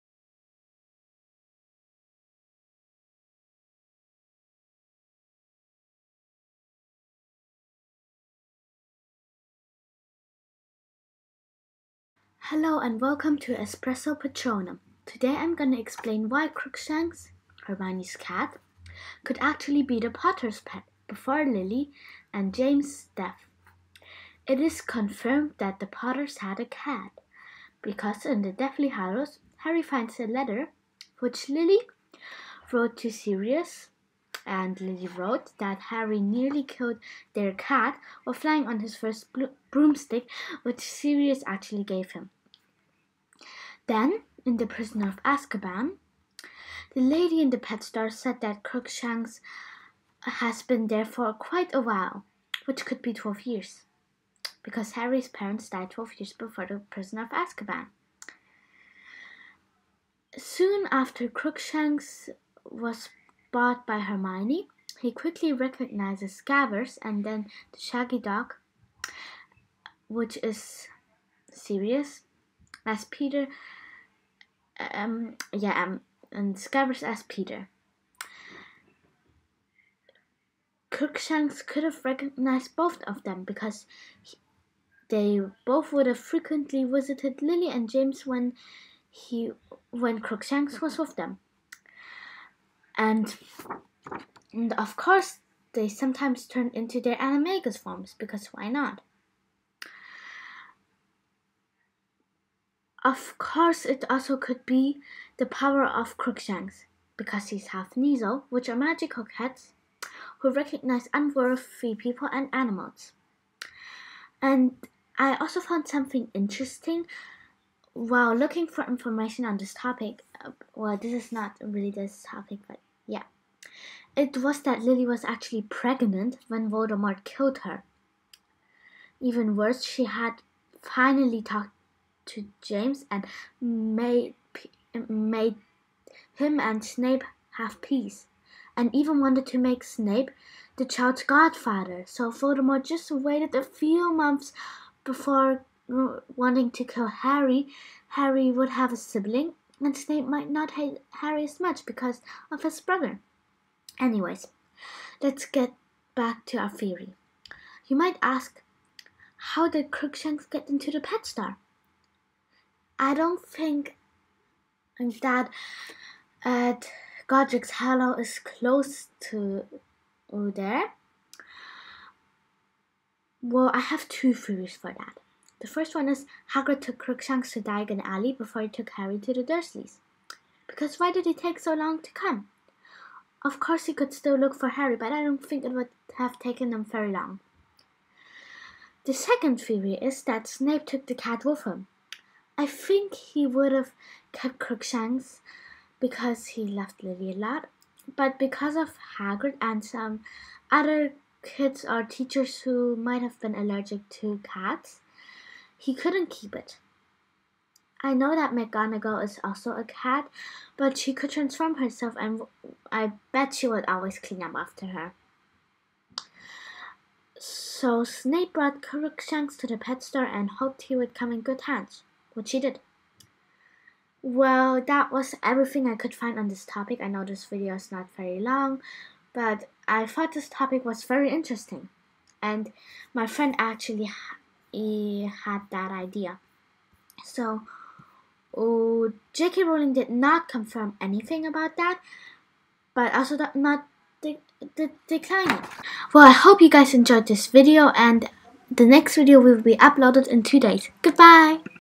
Why Crookshanks could be the Potters' Pet, before Lily and James died
Hey Potterheads!

☕Open for a Message☕
Today I have a special Video!
It is a Theorie (+Voice Reveal(because it was easier than writing everything)).
Why Crookshanks could’ve been Lily and James Pet (before their death)
And everything around that.
It is only 4 Minutes long, but I couldn’t find a lot on that topic.
So sit back and enjoy the video!
☕Espresso Patronum☕

Quote of the day:
“Fear of the name only increases Fear of the thing itself” - Hermione Granger